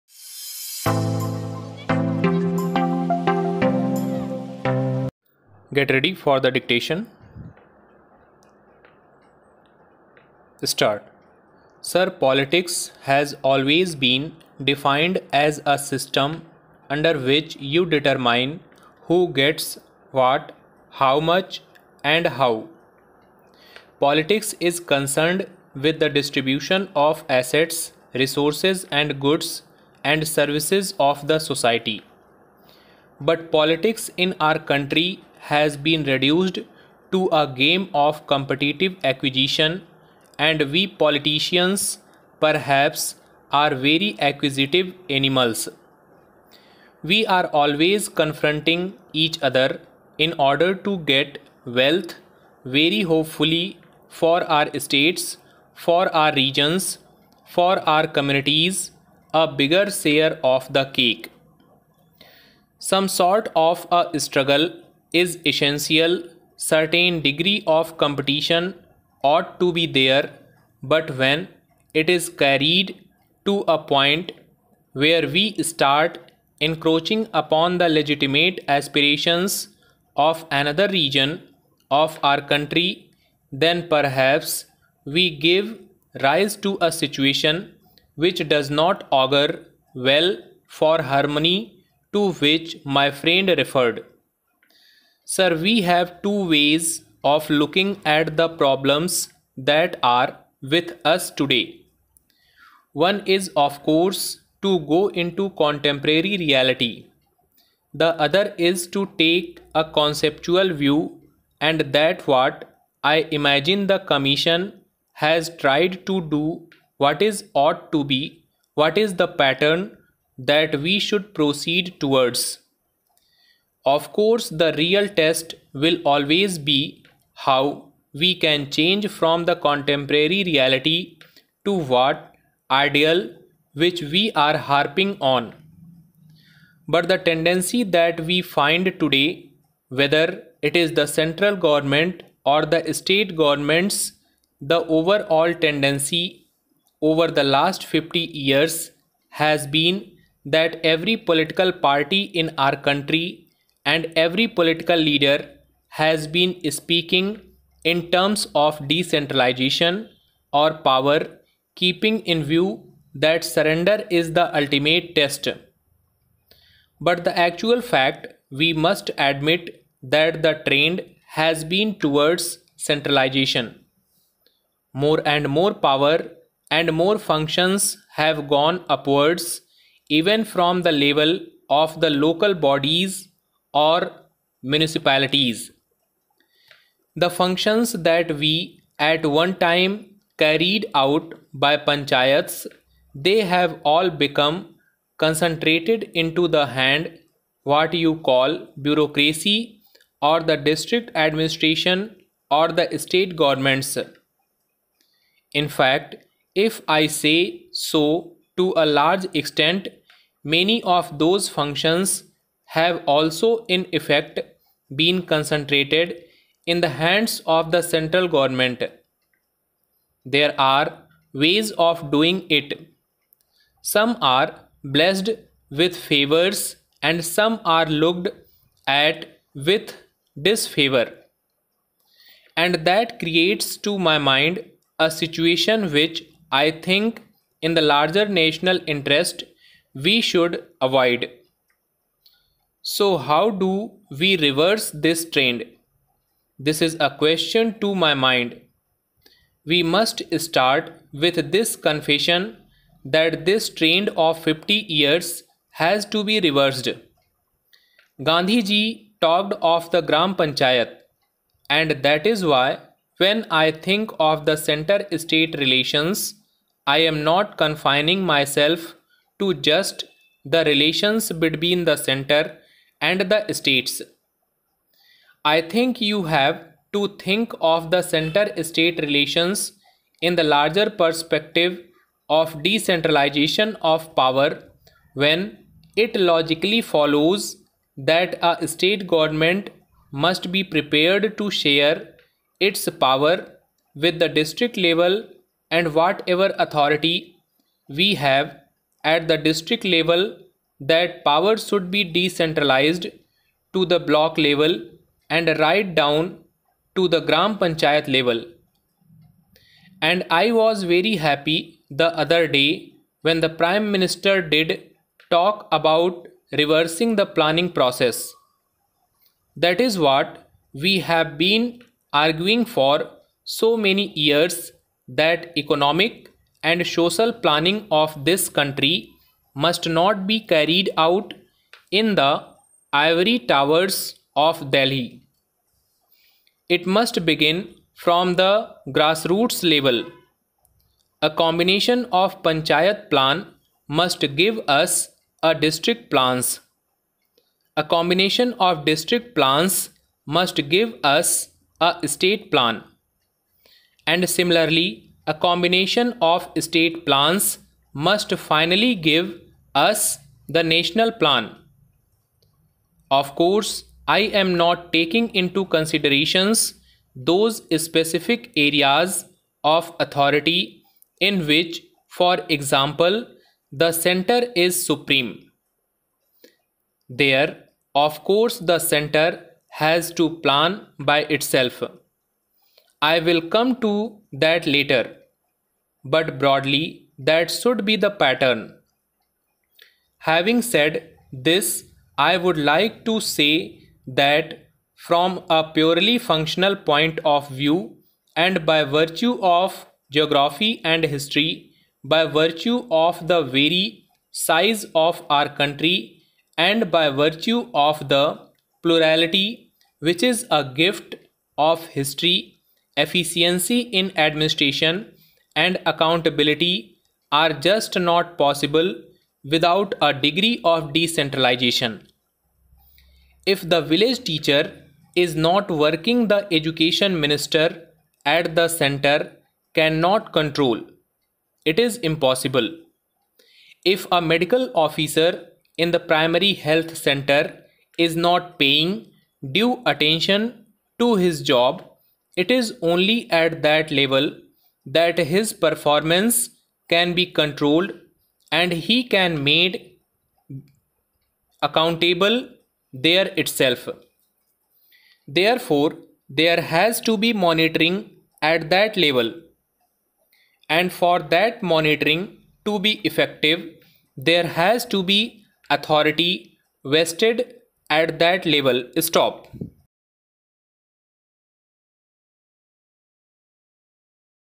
General Progressive Magazine Shorthand Dictation at the speed of 110 words per minute for SSC skill.
This general progressive magazine shorthand dictation recorded at the speed of 110 words per minute to practice for all skill and SSC steno skill test.